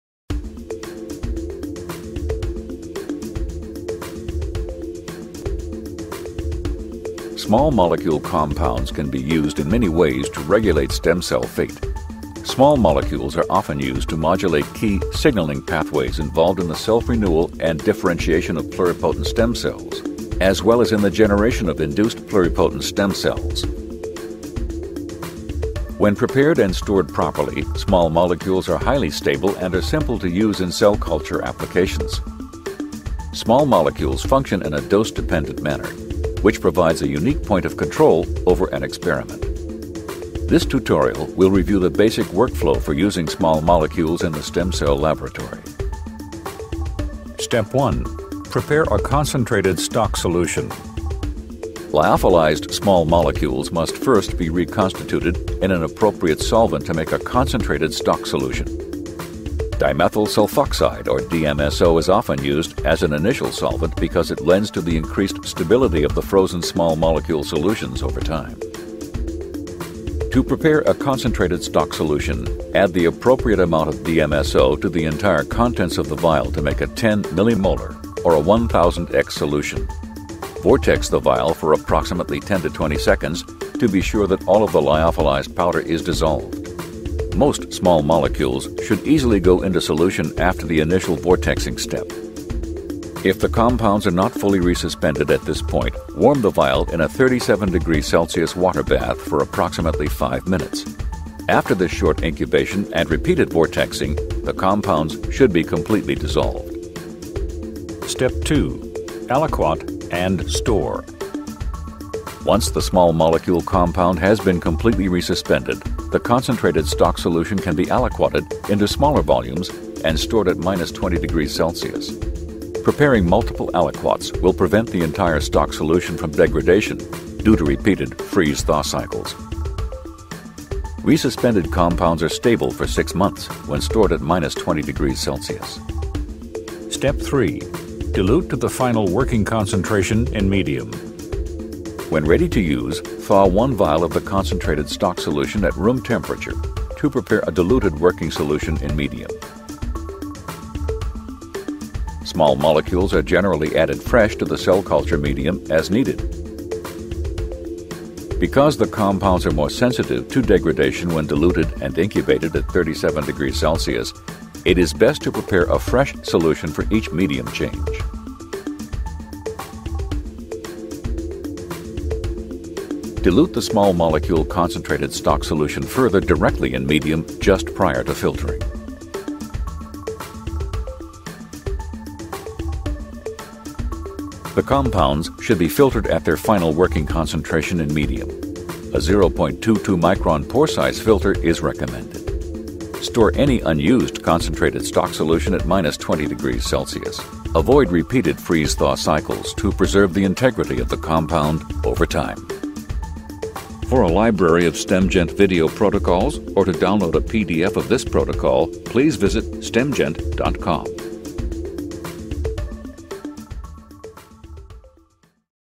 Using Small Molecules For Stem Cell Research | REPROCELL
Learn practical tips for creating a concentrated stock solution

In this explainer video we describe the workflow involved in using small molecules for stem cell research. We go through the steps of preparing a solution in DSMO and then aliquoting and storing the solution for future use.

Got a research project you are looking to outsource? Contact our scientists to arrange a free consultation